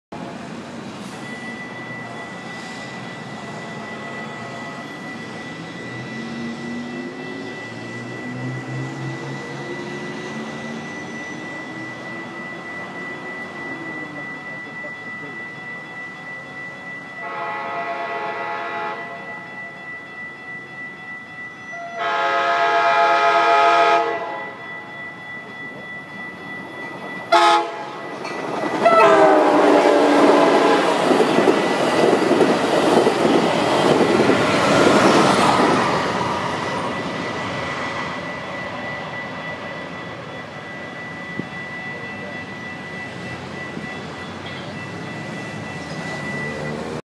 Amtrak 790 rolls toward the Bob Hope Airport Station.. 12-8-12..
AMTK 206 Pushes 790..